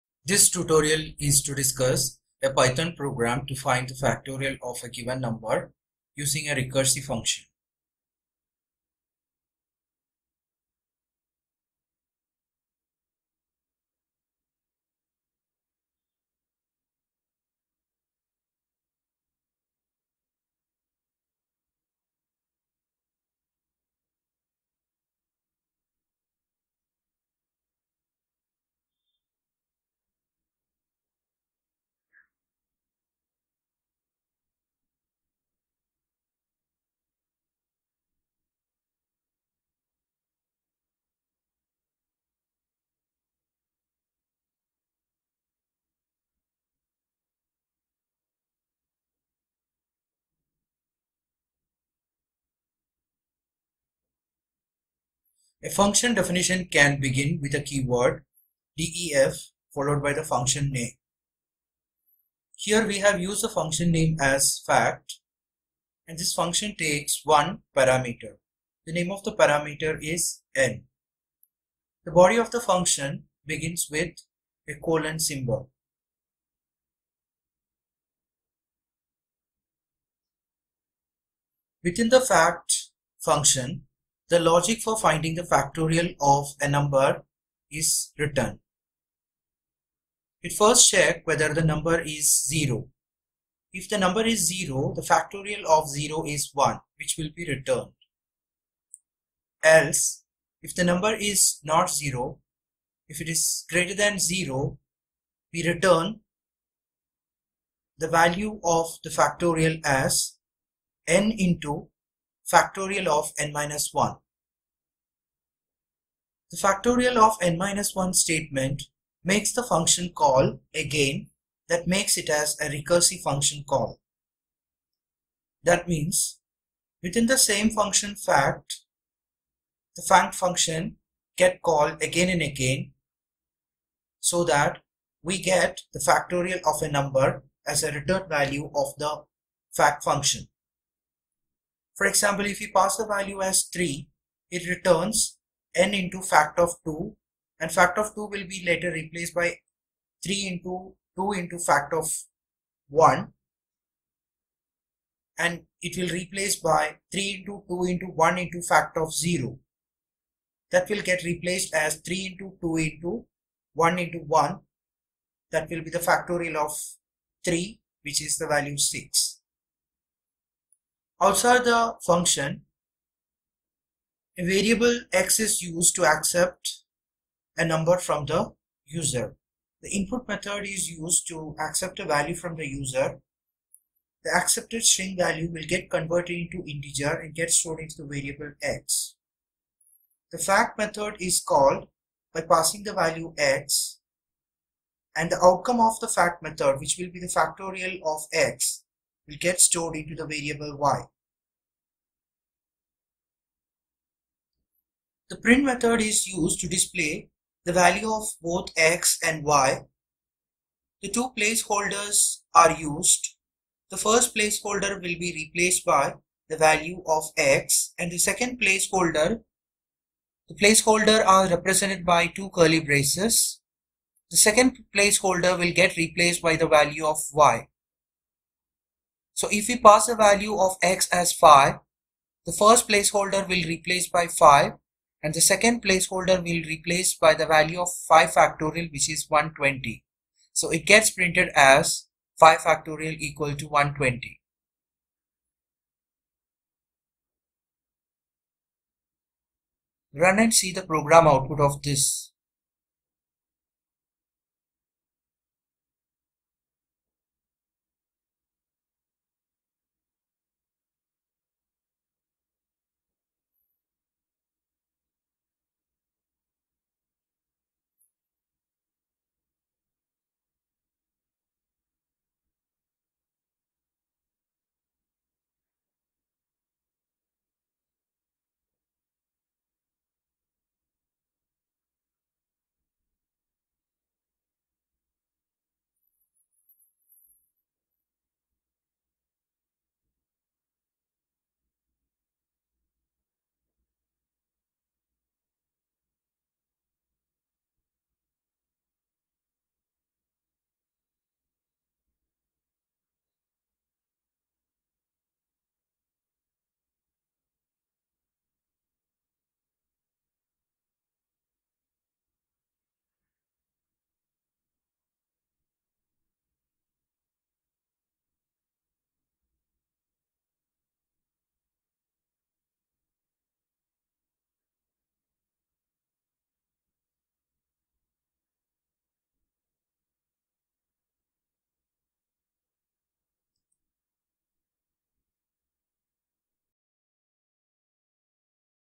Python Program - Factorial of a number
Write a Python program to find the factorial of a given number using recursive function